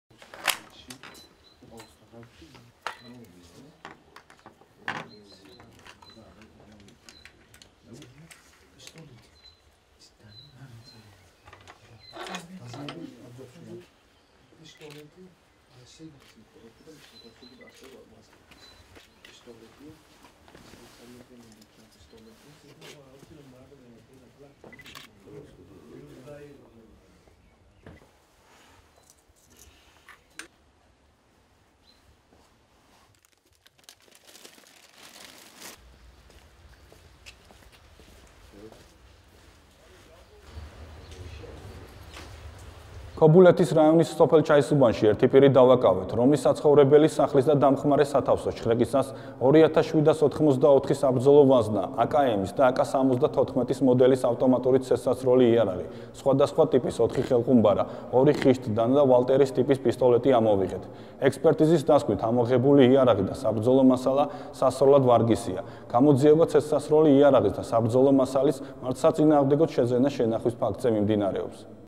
შსს-მ ცეცხლსასროლი იარაღის და საბრძოლო მასალის უკანონო შეძენა-შენახვის ბრალდებით ერთი პირი დააკავა
შსს-ს აჭარის მთავარი სამმართველოს ქობულეთის რაიონული სამმართველოს ჩაქვის პოლიციის განყოფილების თანამშრომლებმა, ჩატარებული ოპერატიულ-საგამოძიებო ღონისძიებების შედეგად, 1977 წელს დაბადებული ზაურ მ. ცეცხლსასროლი იარაღის და საბრძოლო მასალის უკანონო შეძენა-შენახვის ბრალდებით ქობულეთის რაიონის სოფელ ჩაისუბანში დააკავეს.

სამართალდამცველებმა სოფელ ჩაისუბანში, დაკავებულის საცხოვრებელი სახლის და დამხმარე სათავსოს ჩხრეკისას, სხვადასხვა ტიპის იარაღისთვის განკუთვნილი 2784 საბრძოლო ვაზნა, „ა-კა-ემ“-ის და „ა-კა 74“-ის მოდელის ავტომატური ცეცხლსასროლი იარაღი, 25 ავტომატის მჭიდი, სხვადასხვა ტიპის 4 ხელყუმბარა, 2 ხიშტ-დანა და „ვალტერის“ ტიპის პისტოლეტი ამოიღეს. ექსპერტიზის დასკვნით, ამოღებული იარაღი და საბრძოლო მასალა სასროლად ვარგისია. დაკავებული აღიარებს ჩადენილ დანაშაულს.

გამოძიება ცეცხლსასროლი იარაღის და საბრძოლო მასალის მართლსაწინააღმდეგო შეძენა-შენახვის ფაქტზე მიმდინარეობს (საქართველოს სსკ-ის 236-ე მუხლის 1-ლი ნაწილით).